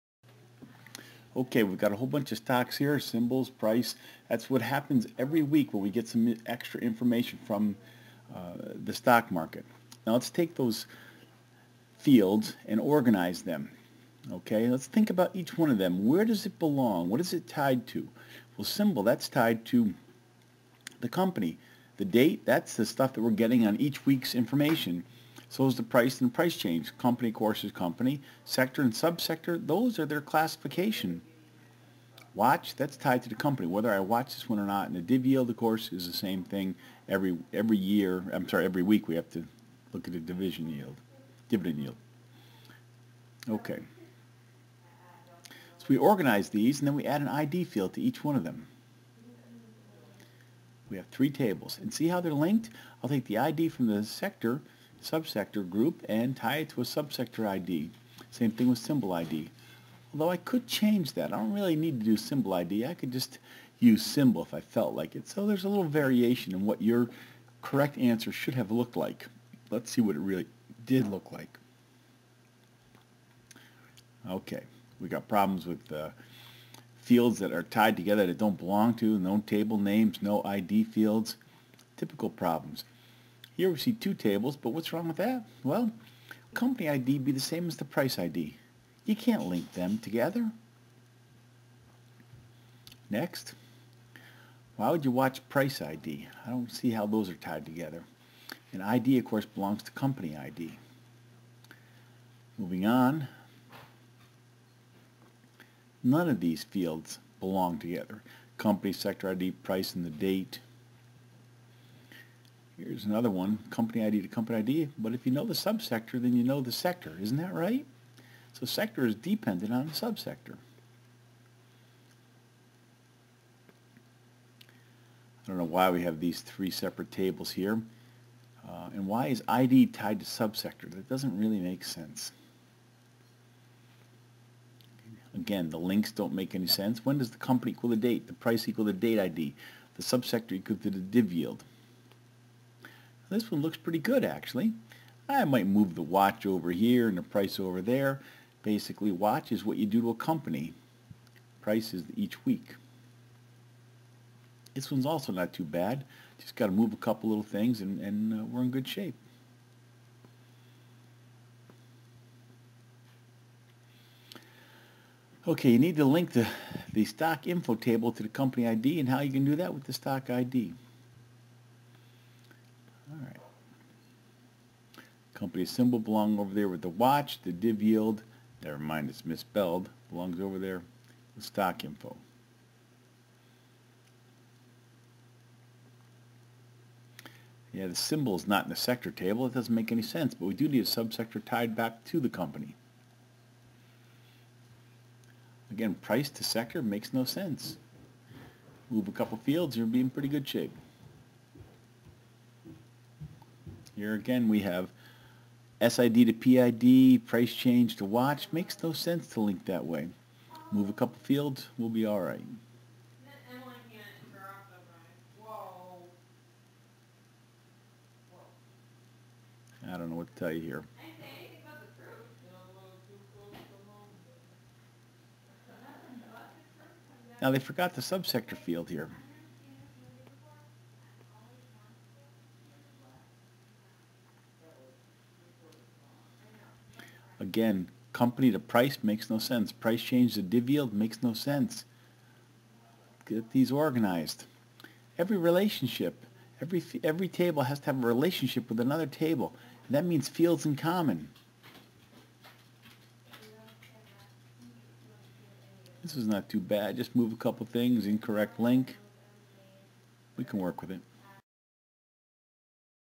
Database: Stock Design Problems
Professor Yunker gives students a task... take a query and normalize the data into tables. In this video, he presents the correct answer and an alternative. Then, he marks up student submissions to demonstrate how a little change here and a little relationship there can turn an incorrect answer into the correct solution.